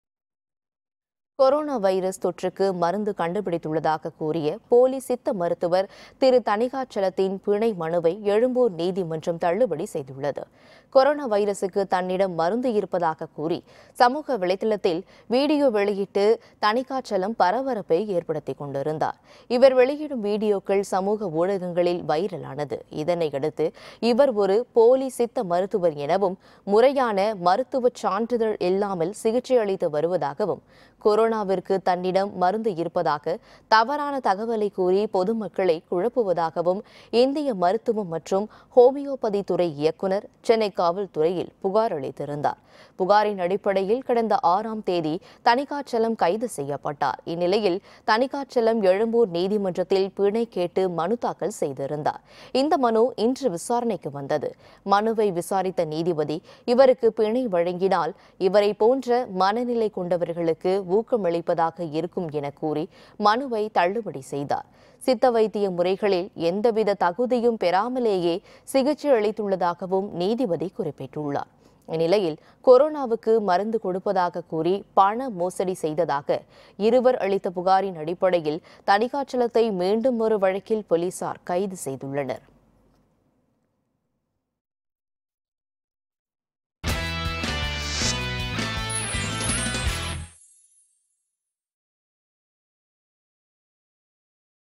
கைதான திருத்தணிகாசலத்தின் பிணை மனு தள்ளுபடி.
கொரோனா வைரஸ் தொற்றுக்கு மருந்து கண்டுபிடித்துள்ளதாகக் கூறிய போலி சித்த மருத்துவர் திருத்தணிகாசலத்தின் பிணை மனுவை எழும்பூர் நீதிமன்றம் தள்ளுபடி செய்துள்ளது.